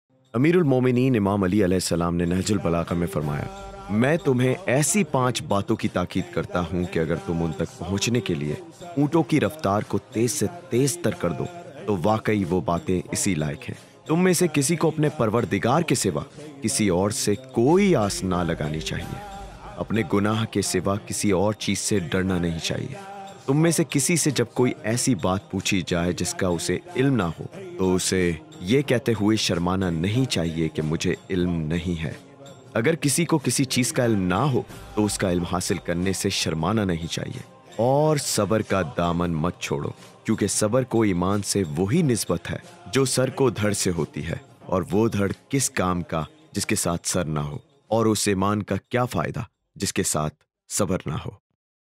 Irshaad No 82 Hazrat Ali (A.S) ki Paanch Baaton ki Takeed | Nehjul Balagah
ایسی پانچ باتیں جن تک پوھنچنے کے لئے اونٹوں کی رفتار کو تیز سے تیز کردیا جائے تو وہ اسی لائق ہیں